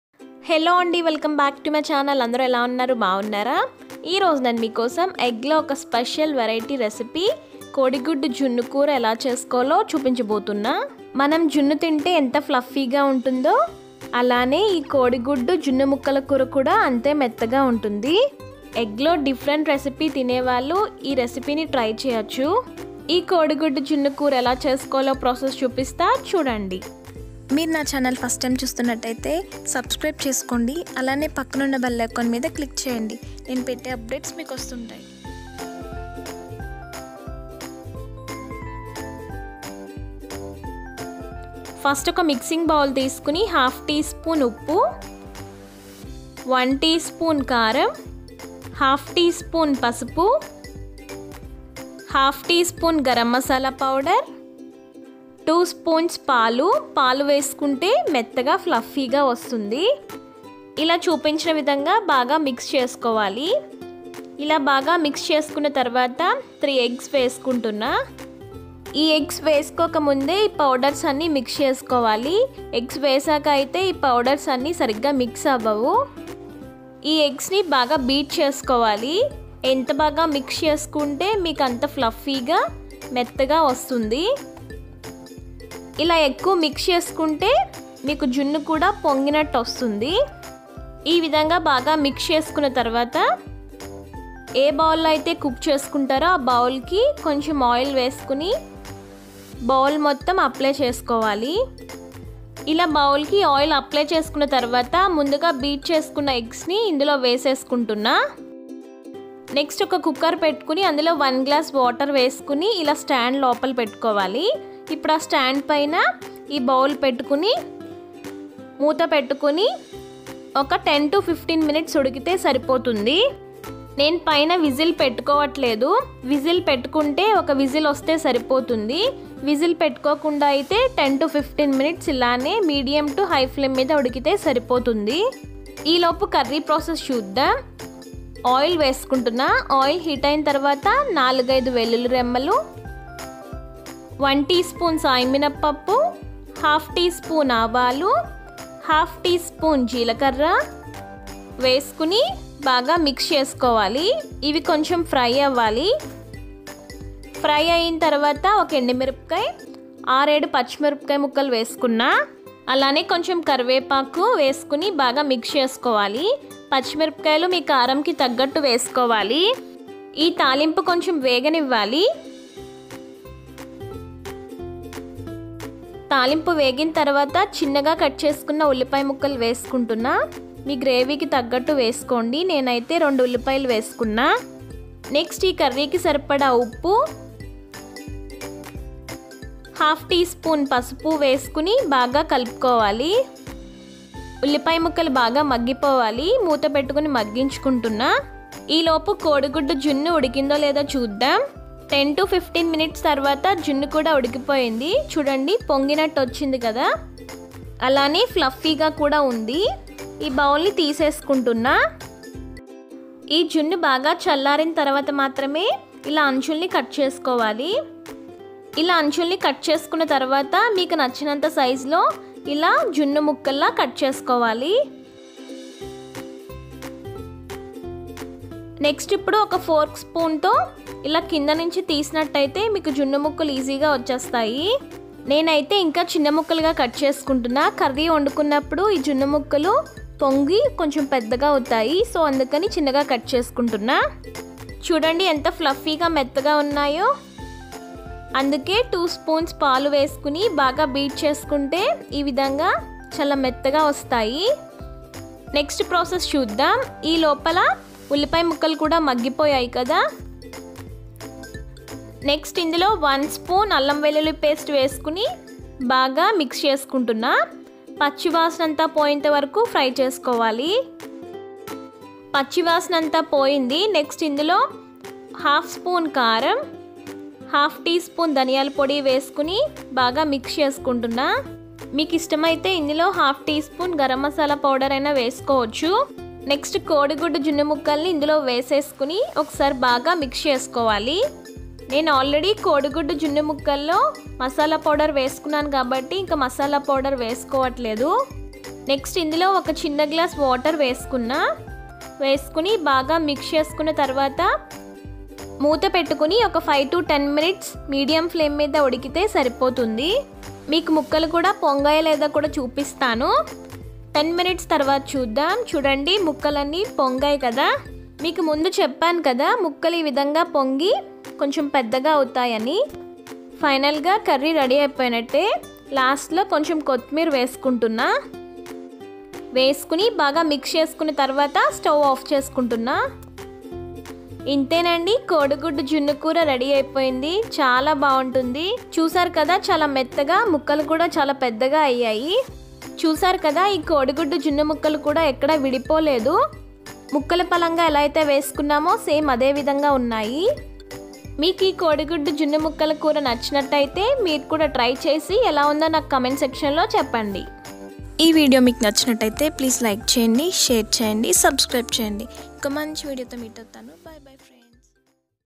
కోడిగుడ్డుతో జున్ను ముక్కలు కూర/జున్నులాగ మెత్తగా,రుచిగా రావాలంటే ఇలా చెయ్యండి/Kodiguddu Junnu Kura/
I'm on Instagram as @mini_many1273. Install the app to follow my photos and videos.

MINI Many Channel